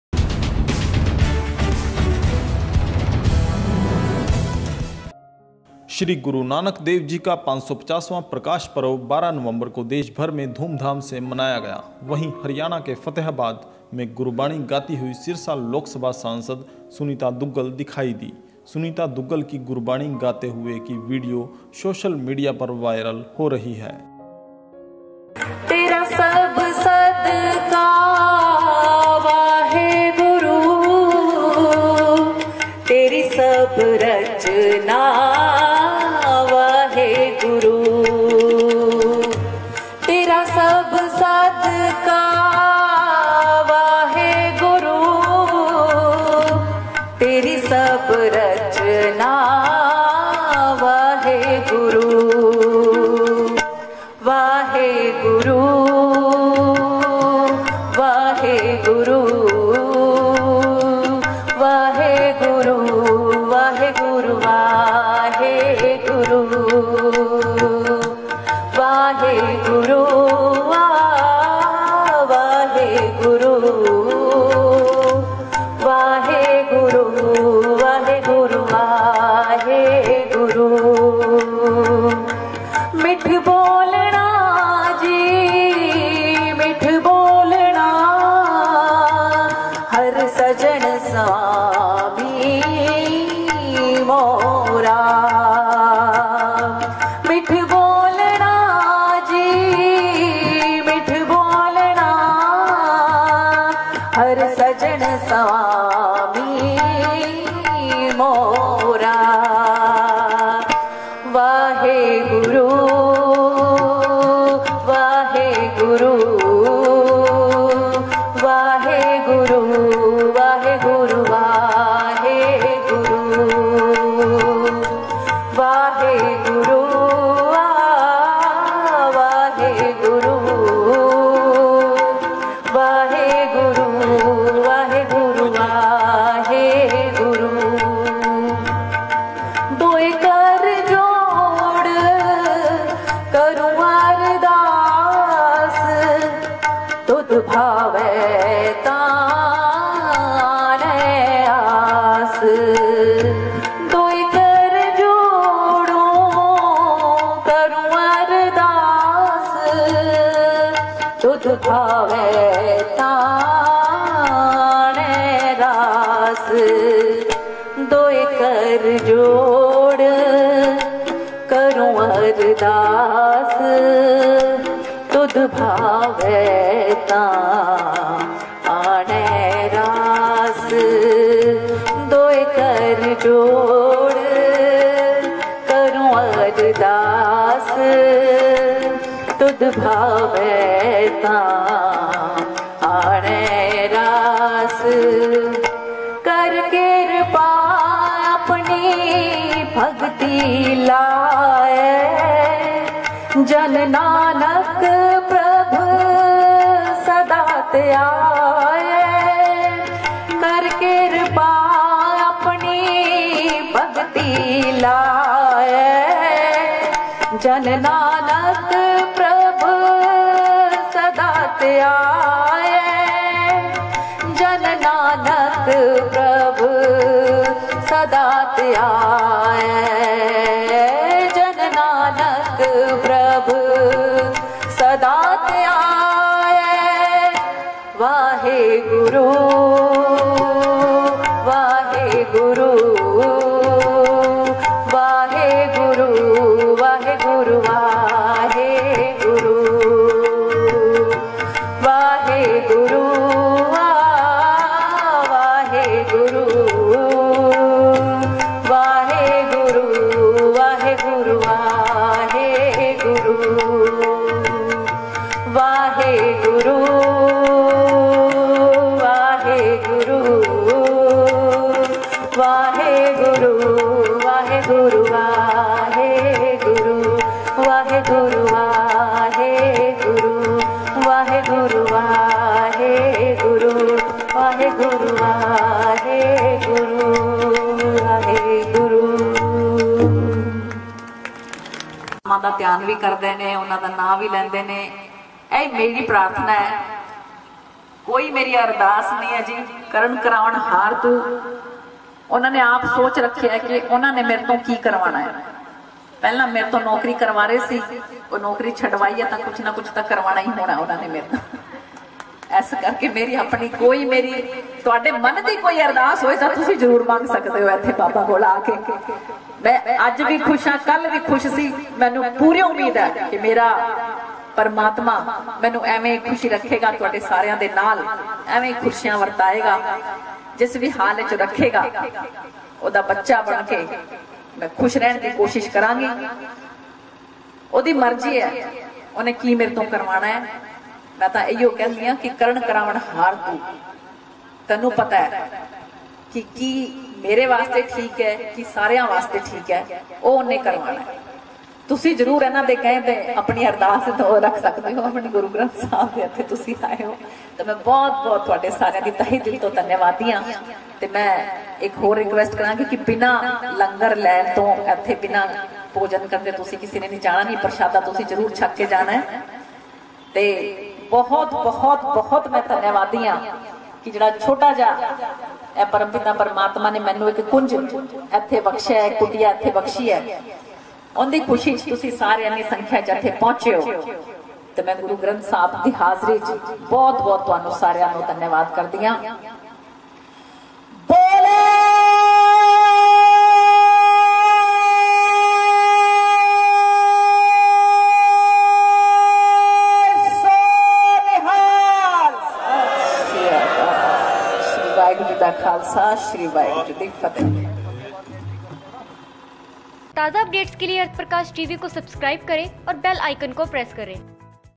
550वें प्रकाशपर्व पर हरियाणा की ये सांसद सुरीली आवाज़ में गुरुबाणी गाती नज़र आई देखें
हरियाणा की ये सांसद 550वें प्रकाशपर्व पर सुरीली आवाज़ में गुरुबाणी गाती हुई नज़र आई देखें